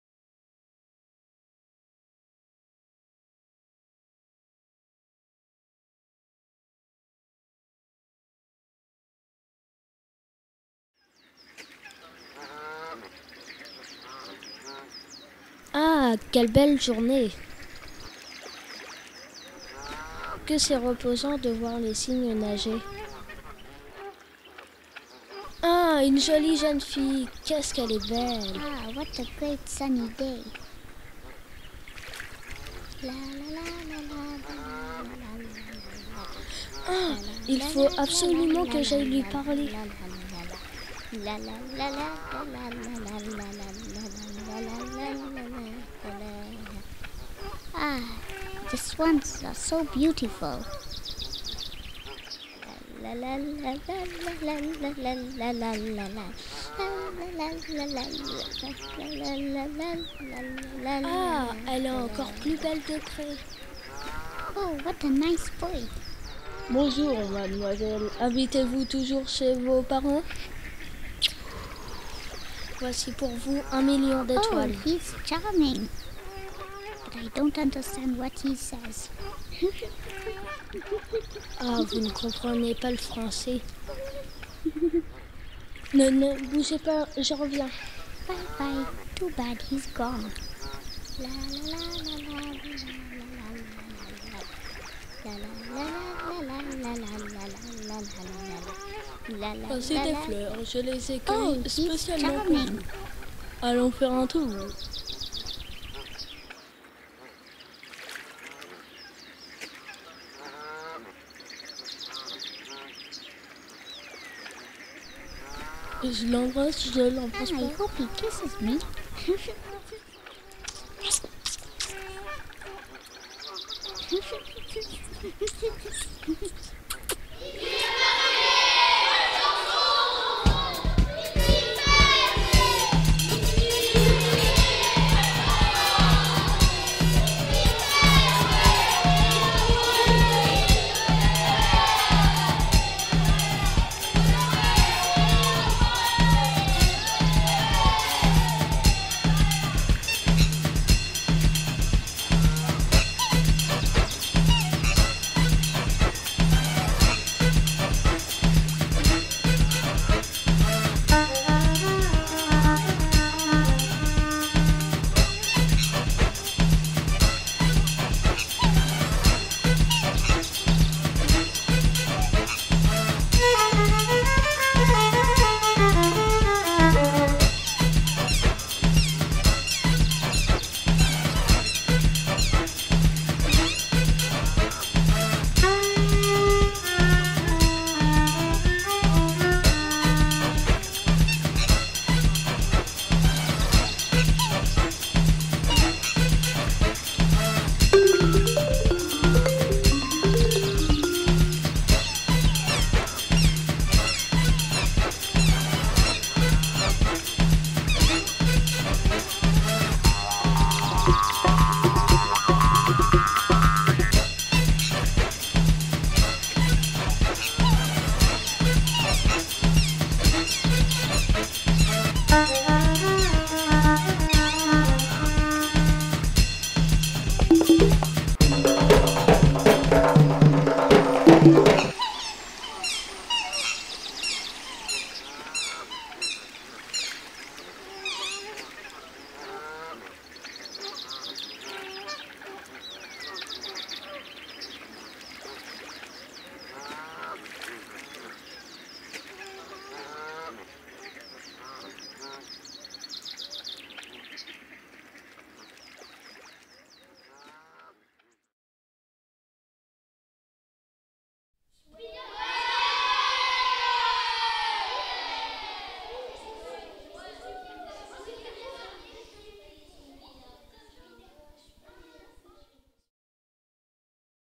Romance.avi
film d'animation réalisé par les élèves de CE2 avec l'aide de Stéphane Lafoy réalisateur
Réalisation dans le cadre de RécréArt Beaune 2012